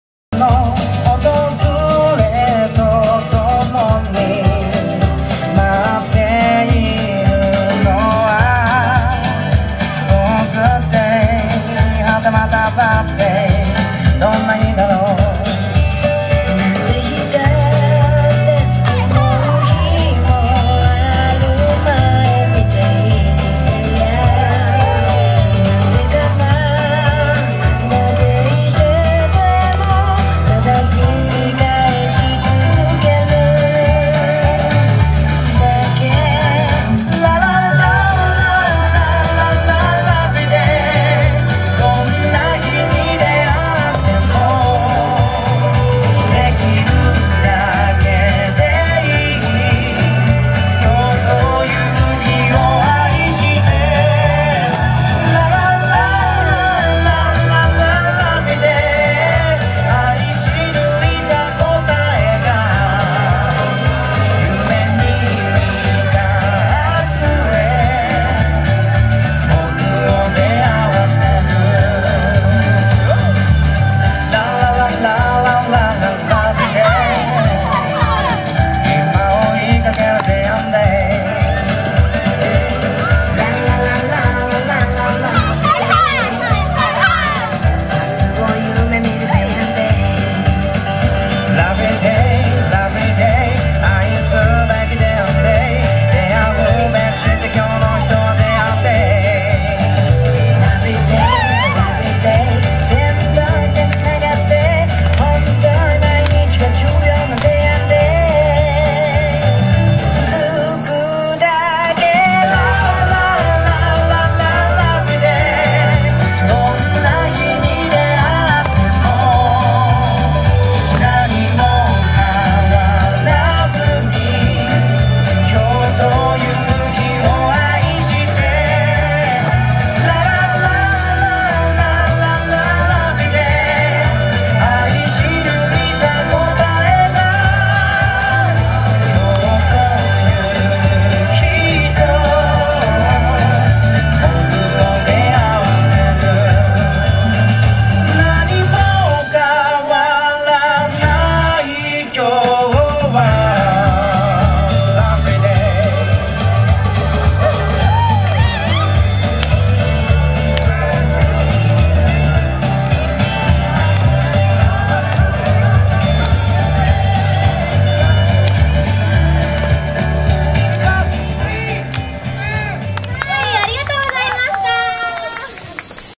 M☆Splash!! ベイタウン朝市-02 2016.6.18
2016年6月18日。幕張ベイタウンで
ベイタウン朝市が開催され
M☆Splash!! が出演しました！

YUKIさん、SHIZUKAさん、
ERIさん、RIKAさんの4名です。